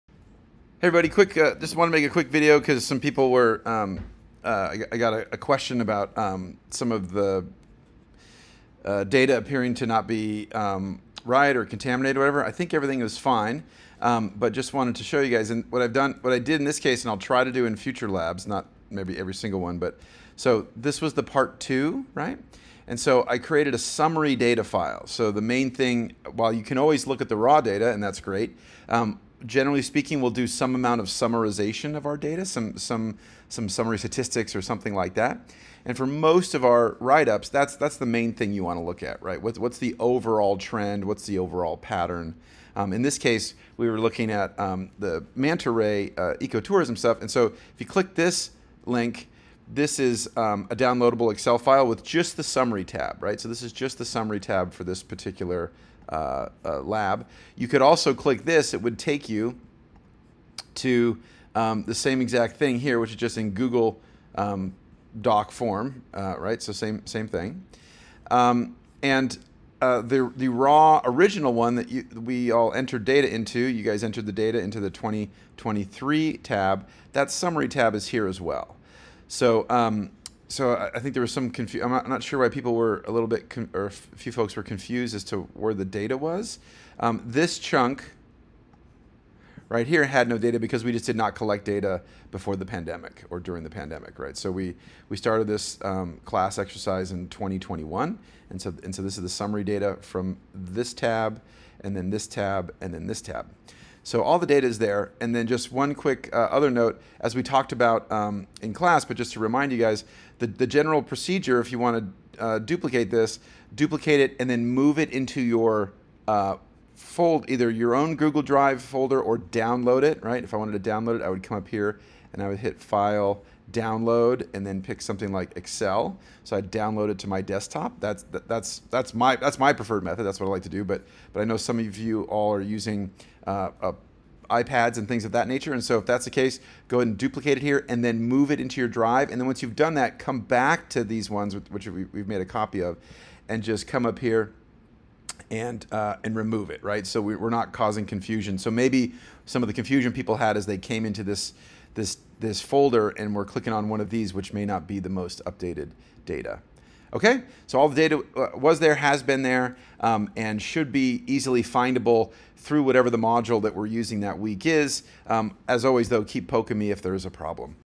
Where to find our class data?
This is a quick review about where to find our collective class data, here using the example of our Manta Ray Lab Part #2.